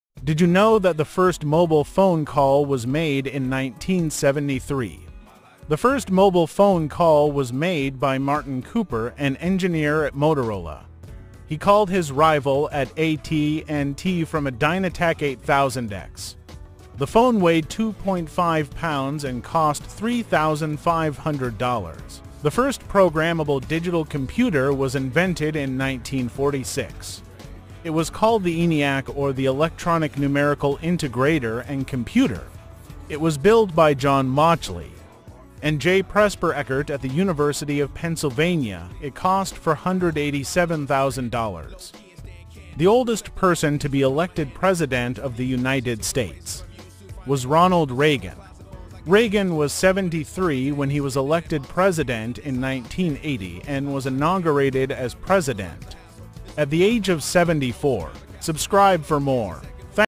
🌟🧠The first mobile phone call. The first digital computer. The oldest president.🌟🧠
Fun Facts 29

What was the first mobile phone call?
When was the first mobile phone call?
Who made the first mobile phone call?
What was the first digital computer?
Who made the first digital computer?
Who was the oldest US president?
Who was the oldest person to be elected?